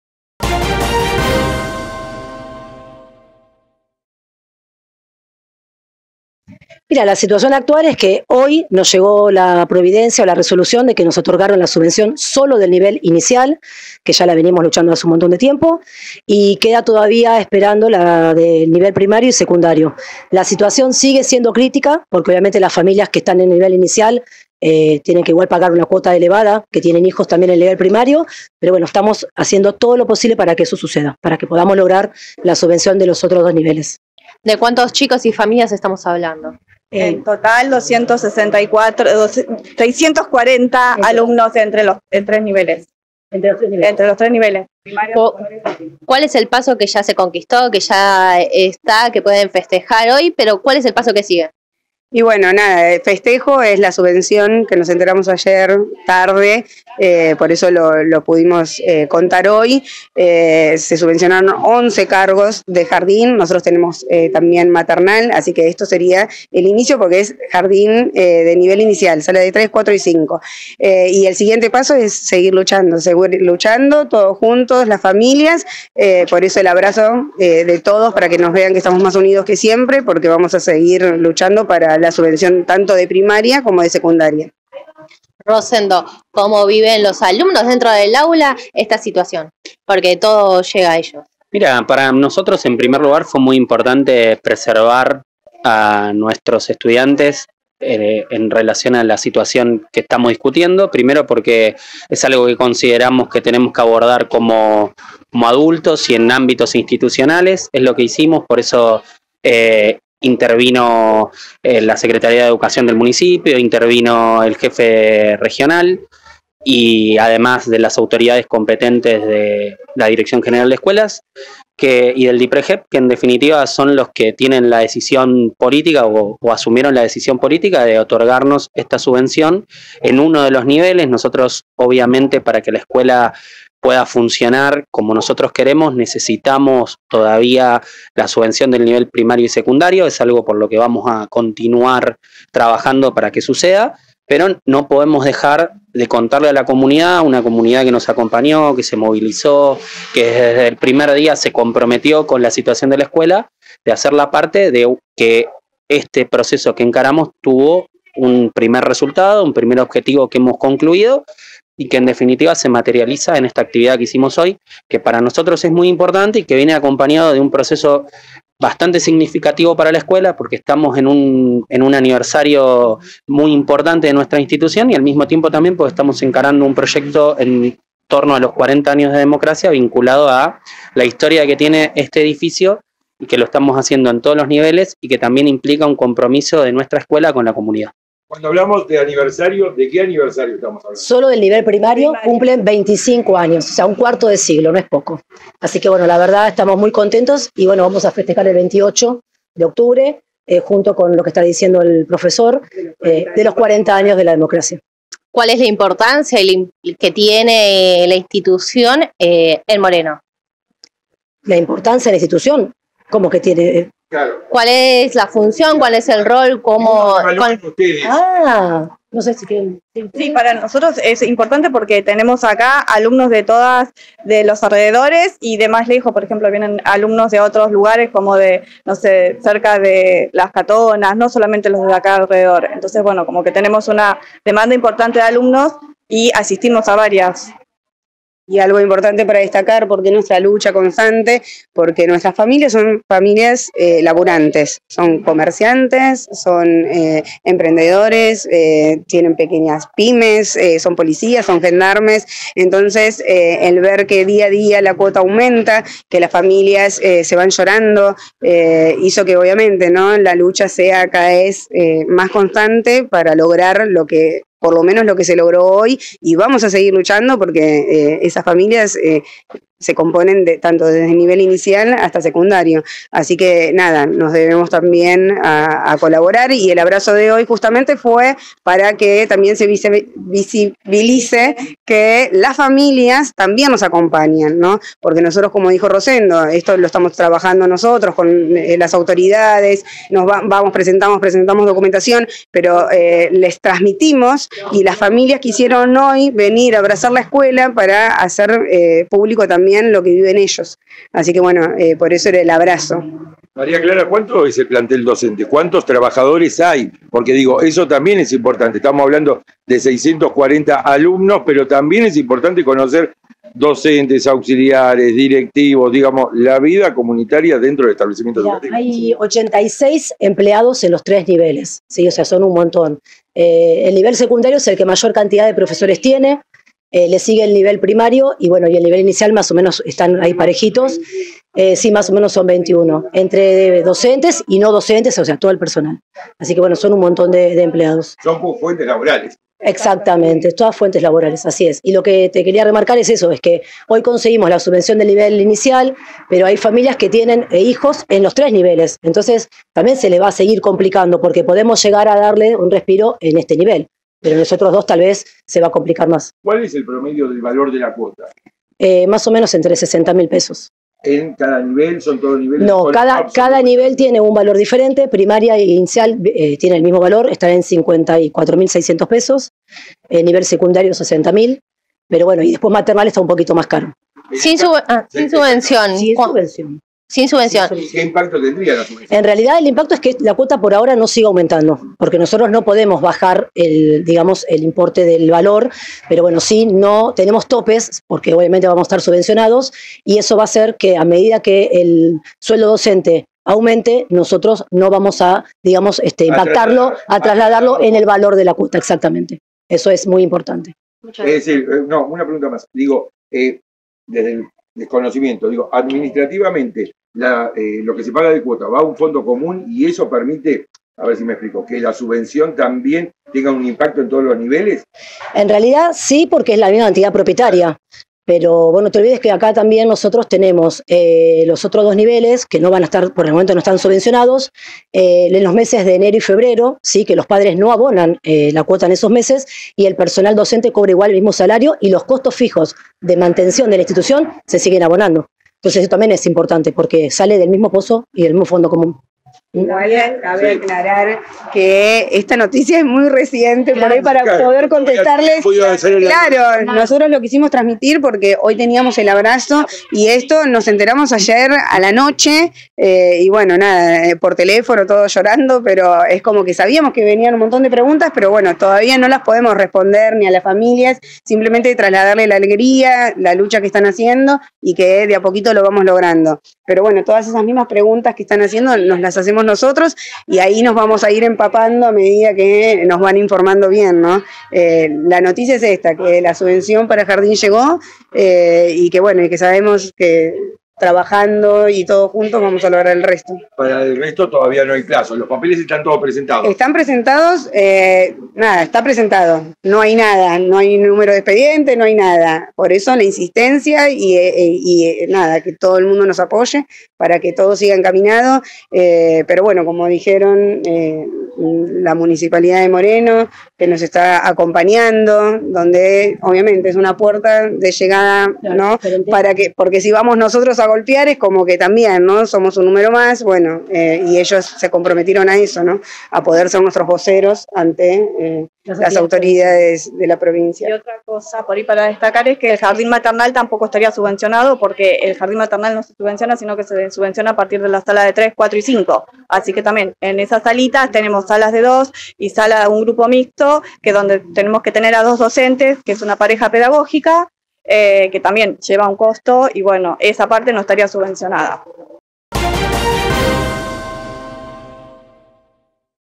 Una buena noticia: El Colegio Amancio Alcorta recibió la subvención del Jardín Mi Casita
Una lucha de años y un paso importante. El Colegio Amancio Alcorta de Moreno centro logró subvencionar el Jardín "Mi Casita". Trabajan para alcanzar el mismo beneficio en la primaria y en la secundaria. Este miércoles la comunidad educativa realizó un abrazo solidario a la institución. Entrevista con Beatriz Borsari (representante legal), Rosendo Martínez (docente), Silvana Rocha (directora del jardín) y María Clara Lombardi (vicedirectora)